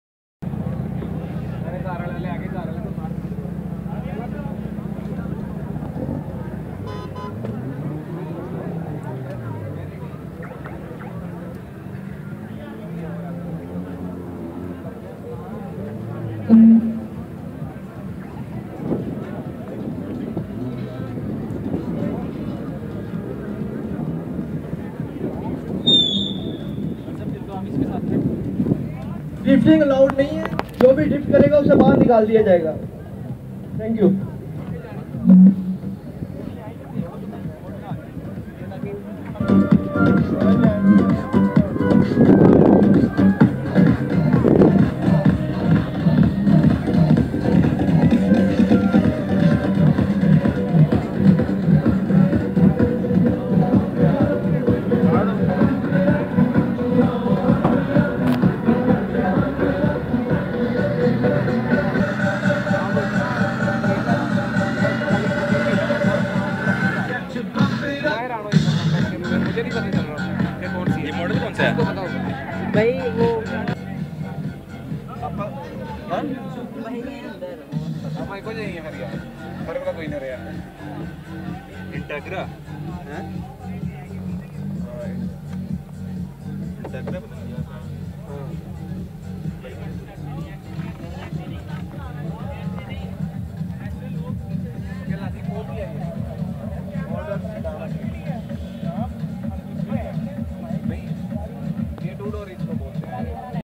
Honda Accord Modified At PGC Motor Show
Honda Accord Modified At PGC Motor Show. It is very old model. It is a property of Tariq Motors. Tariq Motors Count in the Lahore's Largest Rent a car.